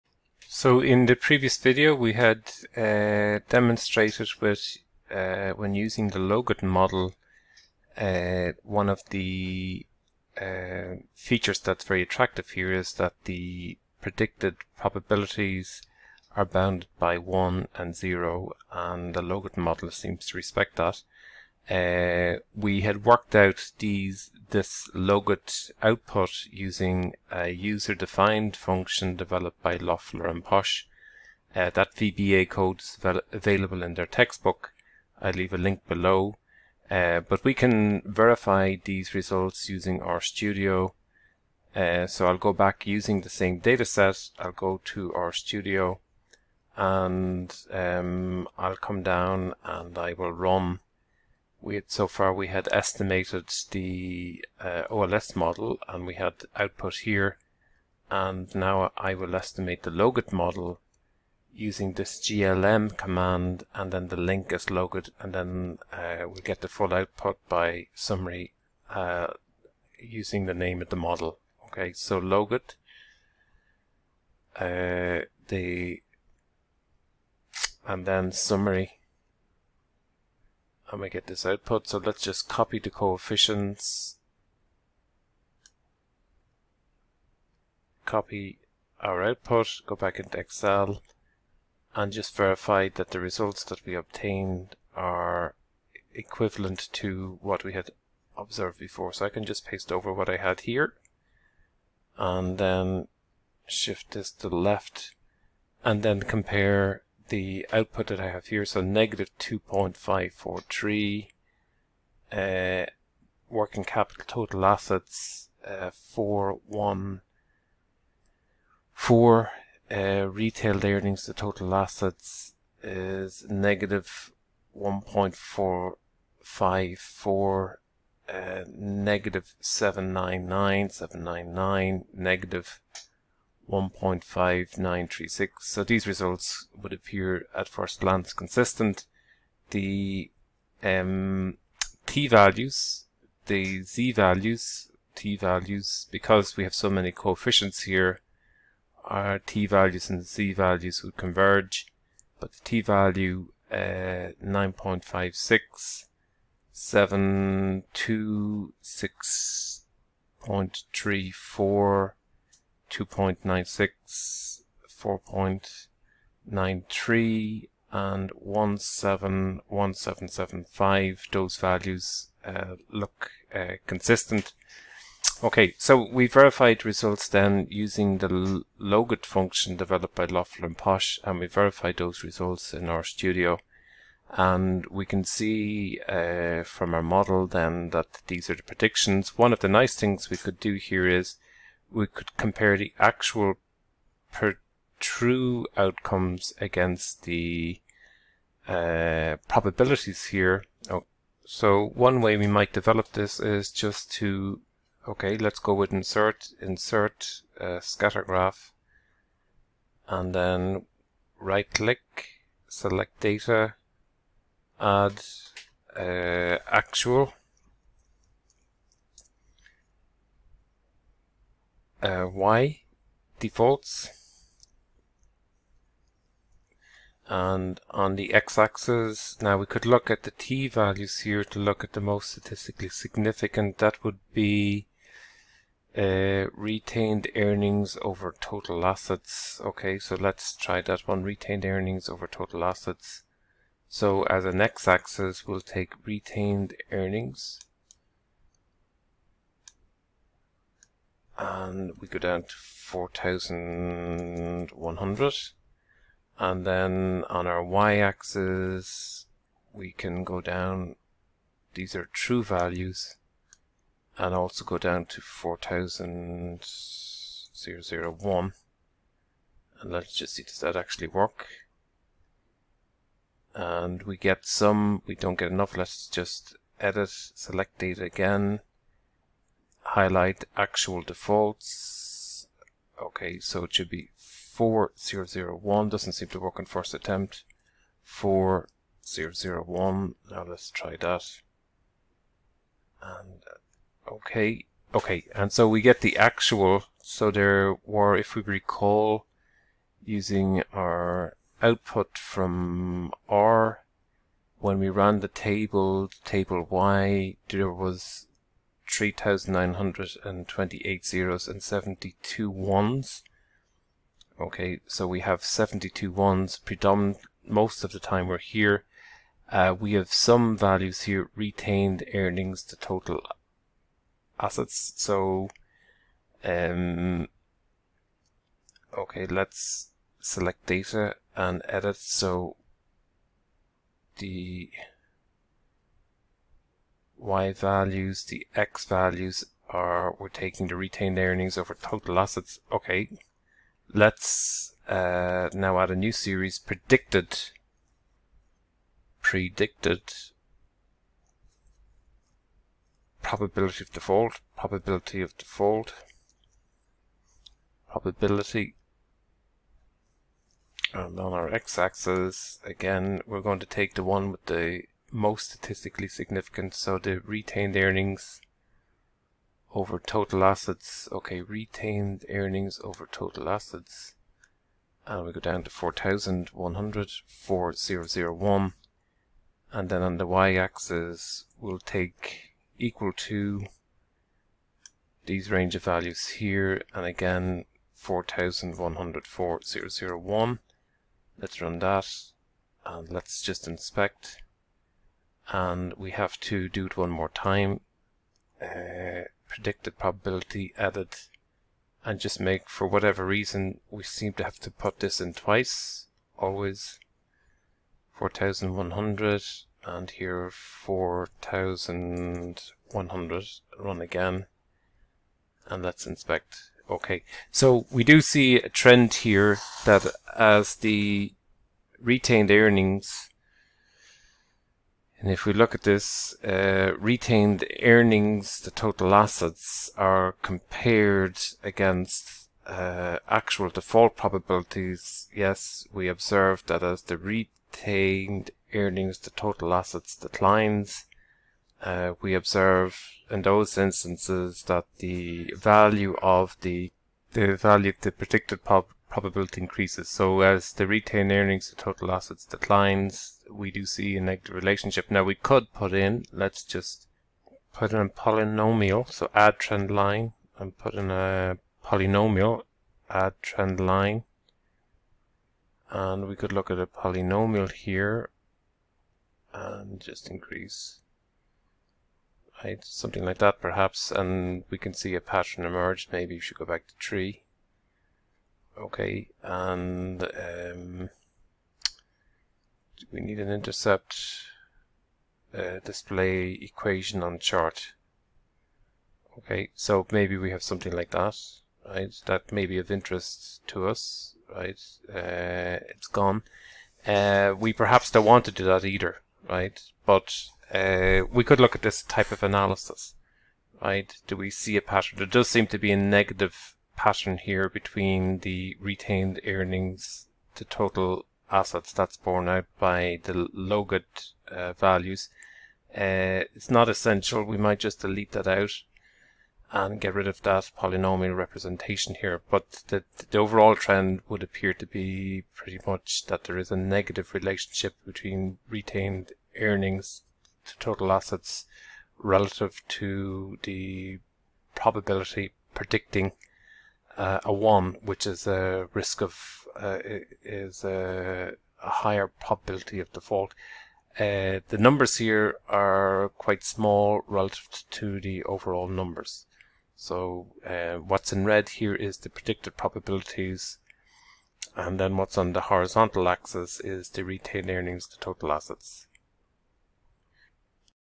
OLS and Logistic Models for predicting Credit Default 3
This analysis of Credit Risk is based on Loeffler and Posch chapter 1:
RStudio is used here to verify results